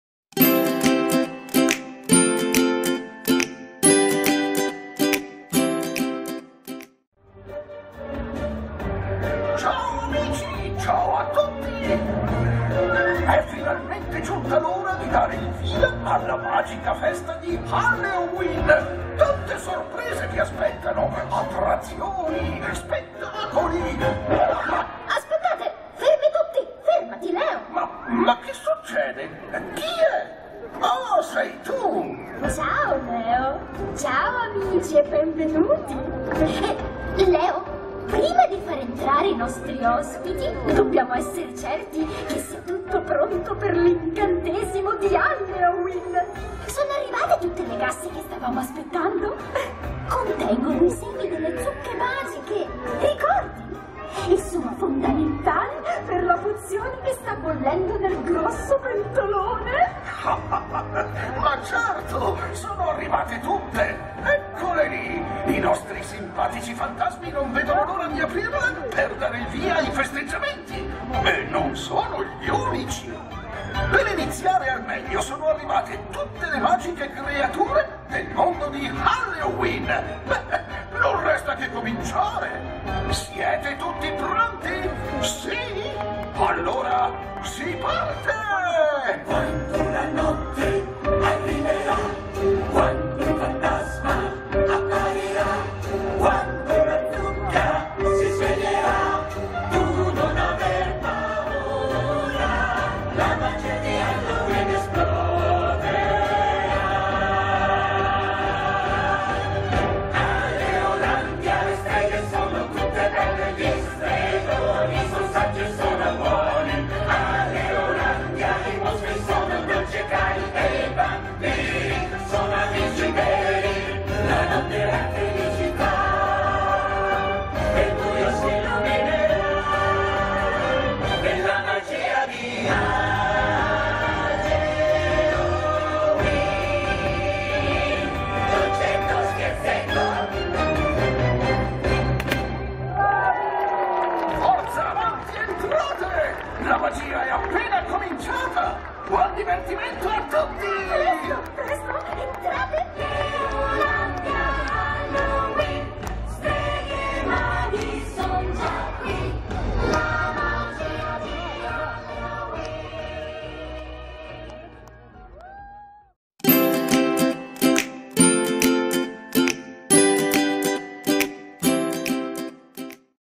Leolandia - Benvenuti a Leolandia: è HalLEOween - 1 ottobre 2016 - unavitaatestaingiu.it
L'inaugurazione della stagione di HalLEOween 2016: Leo Stregone accoglie tutti i LeoOspiti e dà il via alla grande festa di Leolandia: Halloween festoso, scherzetto strepitoso, l'unica festa delle streghe a misura di bambino!